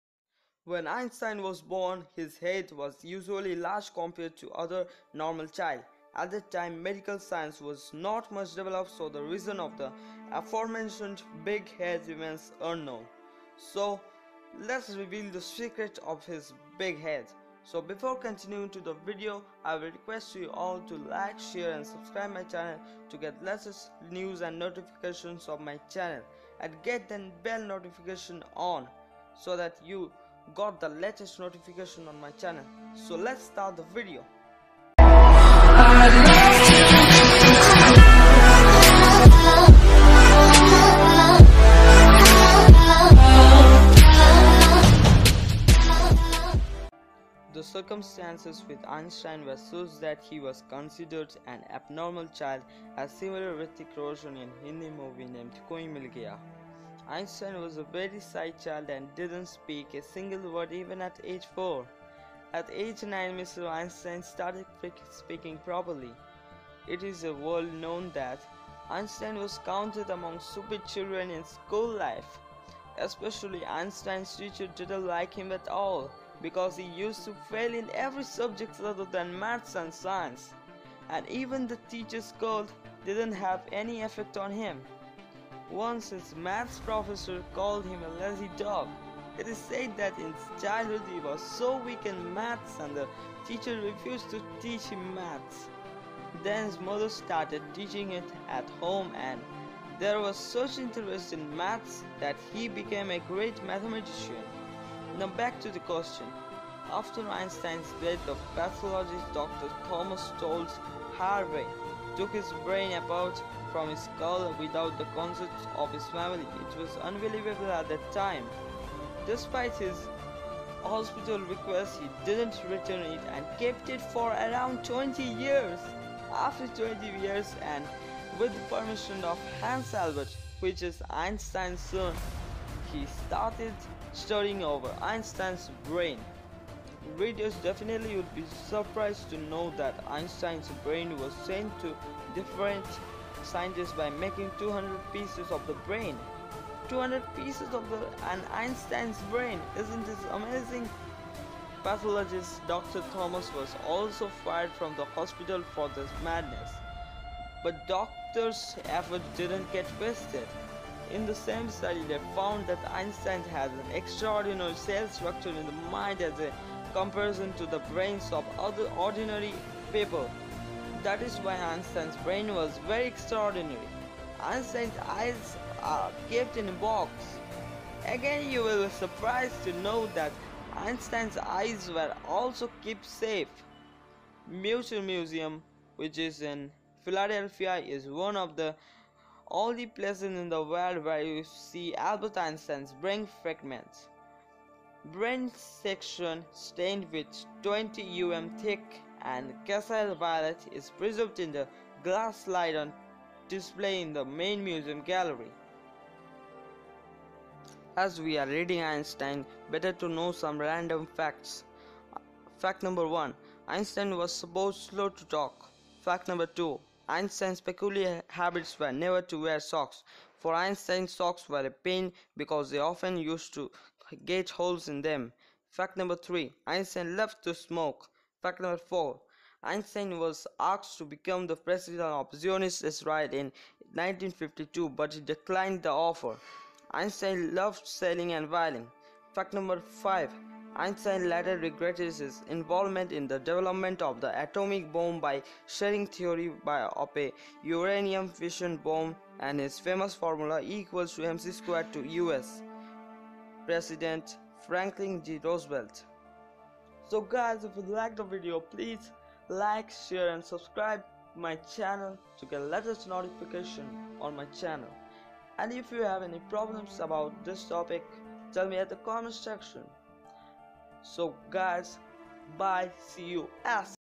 Einstein's Brain Secret:REVEALED(Must Watch)
Hi guys,today I will reveal the brain secret of the great physicist Albert Einstein.

Captions:When Albert Einstein was born, hishead was unusually large compared to other normal children. At that time, medical science was not much developed so the reason for the aforementioned big head remains unknown.

The circumstances with Einstein were such that he was considered an abnormal child as similar Hritik Roshan in Hindi Movie named Koi Mil Gya. Einstein was a very shy child and he did not speak a single word even at age four. At age nine, Mr. Einstein started speaking Properly.[1]

It is a world known that Einstein was counted among stupid children in school life. Especially Einstein's teachers did not like him at all, because he used to fail in every subject other than mathematics and science. And even the teacher's scold did not have any effect on him. Wow, I am not only who ignore teacher’s scold

Once, his mathematics professor called him a lazy dog. It is said that in childhood he was also weak in mathematics and the teacher refused to teach him mathematics. Then his mother started teaching him at home and there was such interest in mathematics that he became a great mathematician.

Now Back to the Question

After Einstein's death, the pathologist Dr. Thomas Stoltz Harvey took his brain apart from his skull without the consent of his family. (It was unbelievable at that time)

Despite the hospital's request he did not return it and kept it for around 20 years. After 20 years, and with the permission of Hans Albert ( Einstein's son), he started studying over Einstein’s brain. Readers definitely would be Surprise to know that Einstein's brainwas sent to different scientists by making 200 pieces of the brain.[2] (200 piece of an Einstein's brain, isn't it amazing)

Pathologist Dr. Thomas was also fired from the hospital for this madness. But doctors' efforts didn't get wasted. In the same study, They found that Einstein had an extraordinary cell structure in the mind as a comparison to the brains of ordinary people. That is why Einstein's brain was very extraordinary. Einstein's eyes are kept in a box.

Again you will be surprised to know that Einstein's eyes are also kept safe.

Mutar Museum[3] (Museum in Philadelphia) is one of the only places in the world where you can see Albert Einstein's brain fragments. Brain section, stained with 20 μm thick and cassyle violet, is preserved in the glass slide on display in the main museum gallery.

As we are reading Einstein, better to know some other random facts.

Einstein was supposedly slow to talk

Einstein's peculiar habits were never to wear socks. For Einstein, socks were a pain because they often used to get holes in them.

Einstein loved to smoke

Einstein was asked to become the President of Zionist Israel in 1952 but he declined the offer.

Einstein loved sailing and violin.

Einstein later regretted his involvement in the development of the atomic bomb by sharing theory of a uranium fission bomb and his famous formula, E=mc² to U.S. President Franklin D. Roosevelt.

If u have an any queries,write in the comment box.